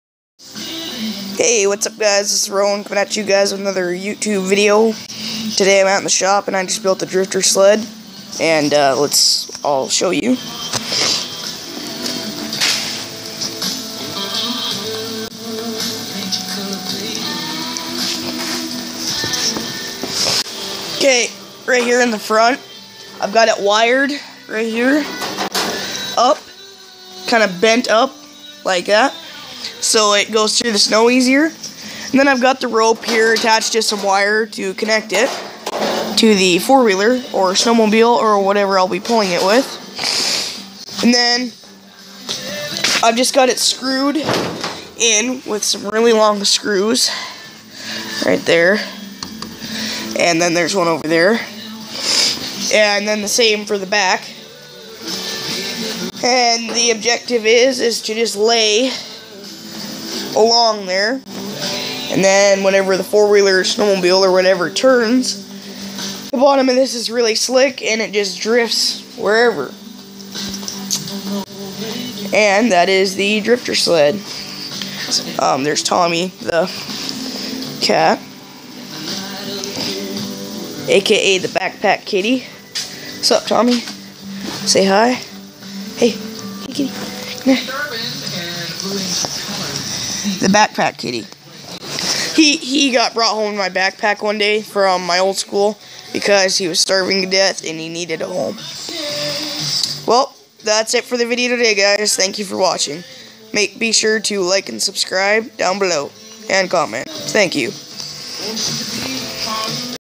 THE DRIFT SLED PART ONE
I have made a drifting sled and this is a vid showing  the finished product with special guest Tommy